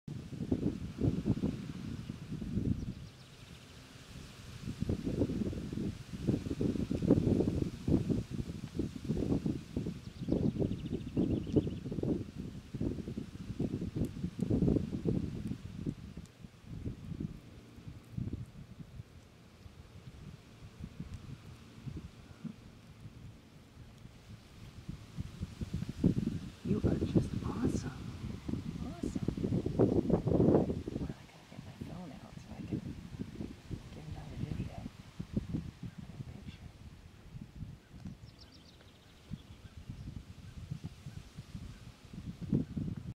Fawn Just Days Old
A White Tale Deer Fawn strolled up to me as I was mowing my lawn.  You could still see it's umbilical cord.  It was just days old.